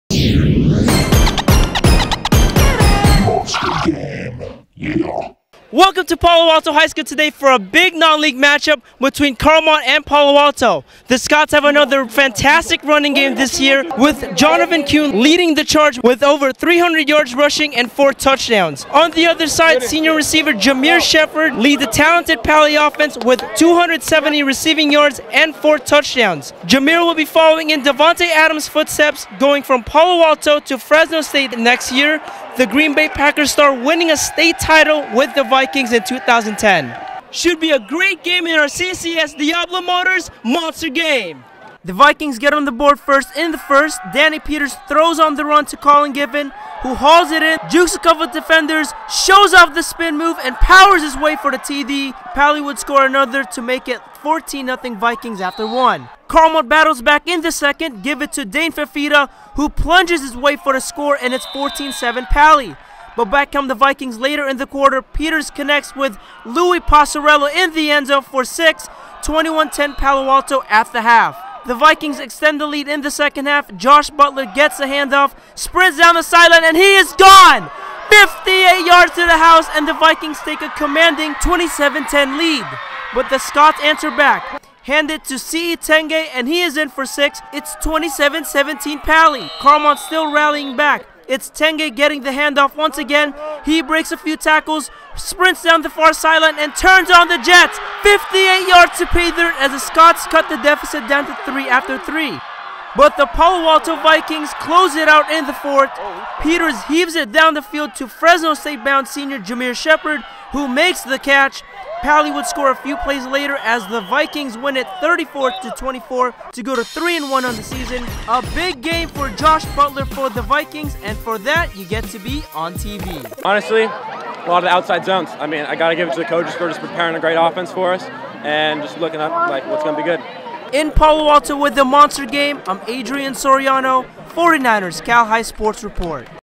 Carlmont at Palo Alto Football | Danny Peters vs Dane Fifita 09 20 19
THE VIKINGS GET ON THE BOARD FIRST IN THE FIRST...DANNY PETERS THROWS ON THE RUN TO COLIN GIFFEN WHO HAULS IT IN THEN JUKES ONE DEFENDER, JUKES ANOTHER,  SHOWS OFF THE SPIN MOVE AND POWERS HIS WAY ALL THE WAY FOR THE TD...PALY WOULD SCORE ANOTHER TD LATER ON AND IT'S 14-0 VIKES AFTER 1

CARLMONT BATTLES BACK IN THE SECOND...GIVE IT TO DANE FIFITA WHO PLUNGES HIS WAY FOR THE SCORE AND IT'S 14-7 PALO ALTO

BUT BACK COME THE VIKINGS LATER IN THE QUARTER...PETERS CONNECTS WITH LOUIS PASSARELLO IN THE ENDZONE FOR THE TD...21-10 VIKES AT THE HALF

PALY EXTENDS THE  LEAD IN THE SECOND HALF...JOSH BUTLER GETS THE HANDOFF, SPRINTS DOWN THE SIDELINE AND HE IS GONE...58 YARDS TO THE HOUSE AND THE VIKINGS TAKE A COMMANDING 24-10 LEAD

BUT THE SCOTS ANSWER BACK...HAND IT TO SI'I TENGEI AND HE IS IN FOR SIX...24-17 PALY

CARLMONT STILL RALLYING BACK..IT'S TENGEI GETTING THE HANDOFF AGAIN, HE BREAKS A FEW TACKLES, SPRINTS DOWN THE FAR SIDELINE AND TURNS ON THE JETS...58 YARDS TO PAYDIRT AS THE SCOTS CUT THE DEFICIT DOWN TO THREE AFTER THREE

HOWEVER THE VIKINGS PUT THE FINISHING TOUCHES IN THE FOURTH...PETERS HEAVES IT DOWN THE FIELD TO FRESNO STATE BOUND SENIOR JAMIR SHEPARD WHO MAKES THE CATCH...PALY WOULD SCORE A FEW PLAYS LATER AS THE VIKINGS WIN IT 34-24 TO GO TO THREE AND ONE ON THE SEASON...WE CAUGHT UP WITH JOSH BUTLER AFTER THE GAME.